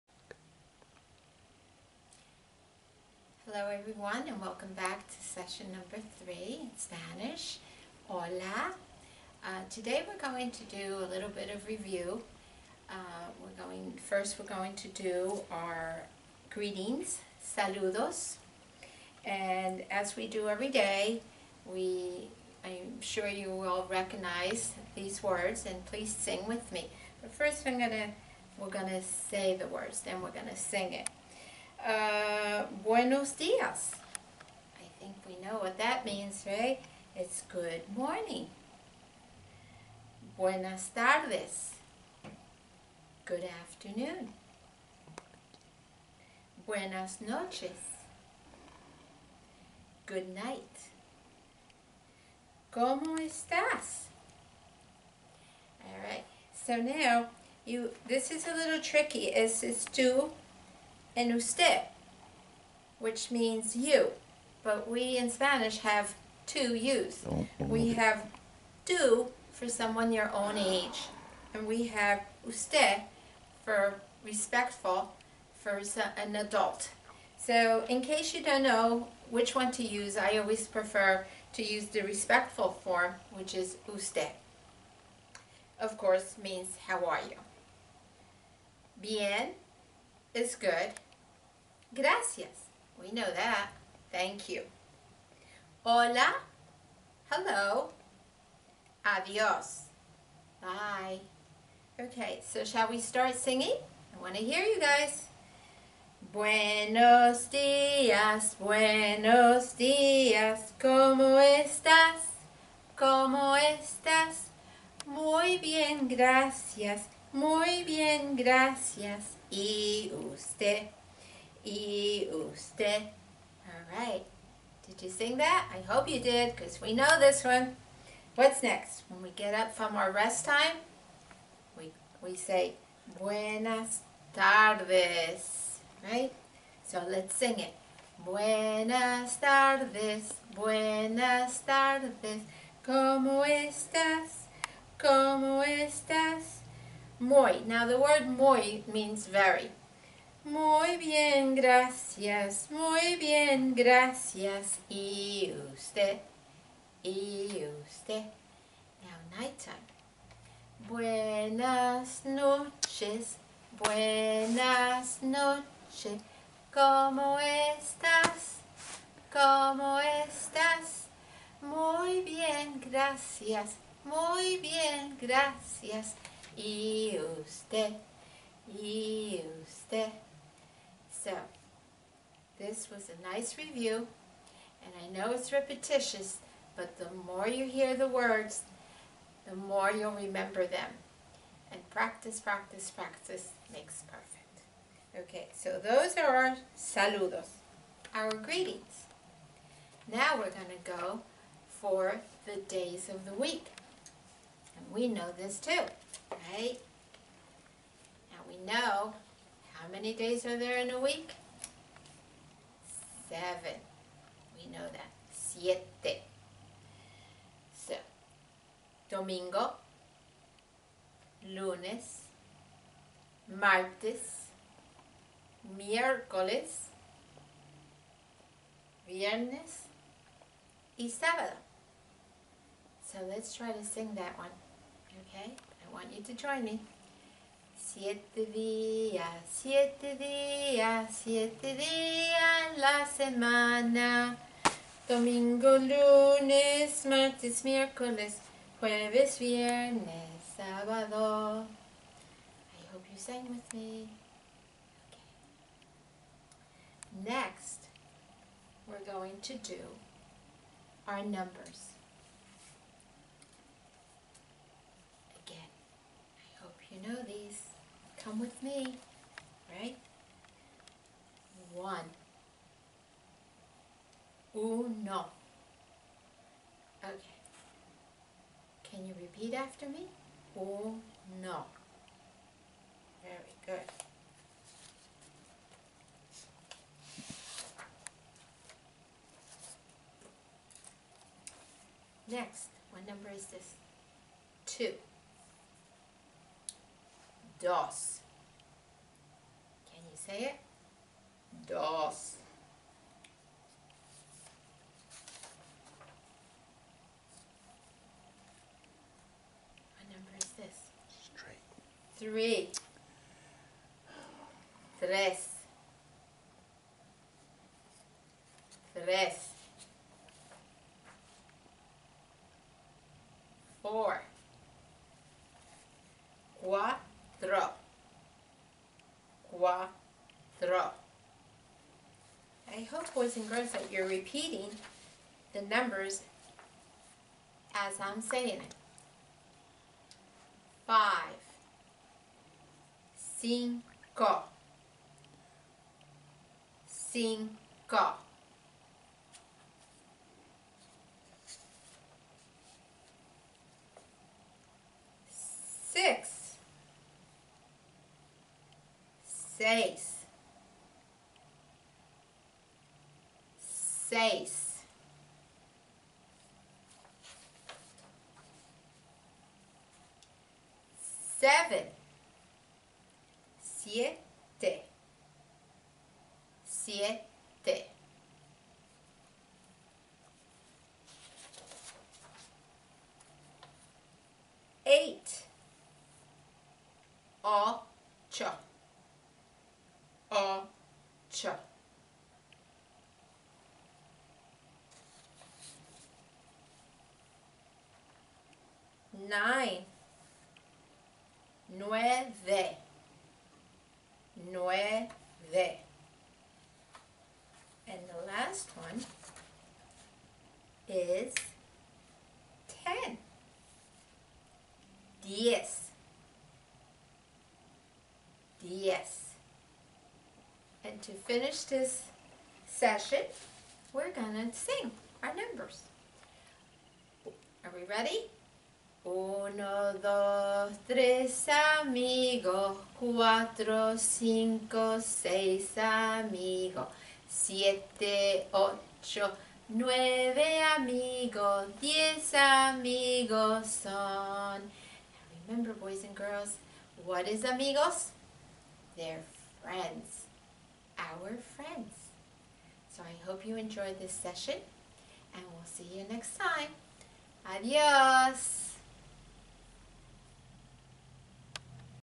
Spanish Class 4/21/20
Review - Numbers, Greetings, Days of the week